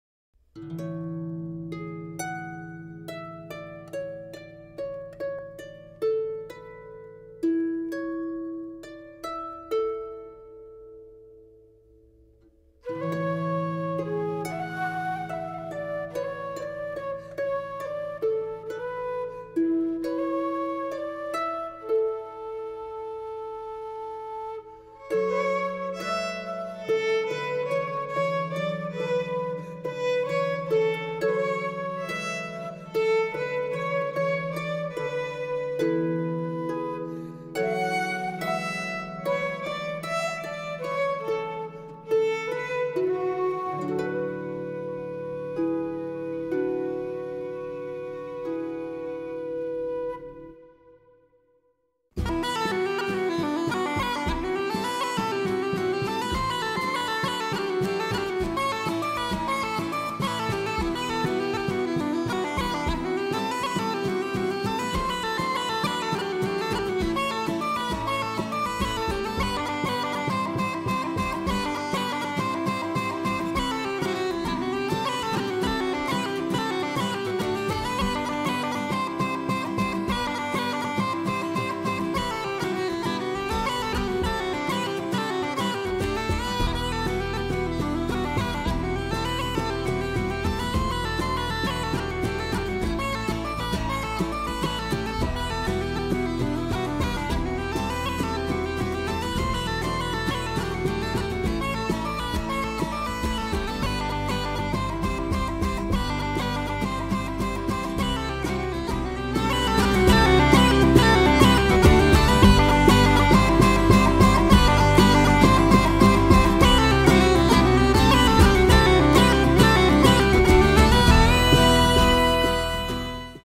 Archie the Wheaten Terrier in "Riverdance"
Archie once saw a lizard run into this plant and he has being trying to "scare" it out ever since.  He likes to do a little "dance" around the plant ...until Gracie comes over and tries to play!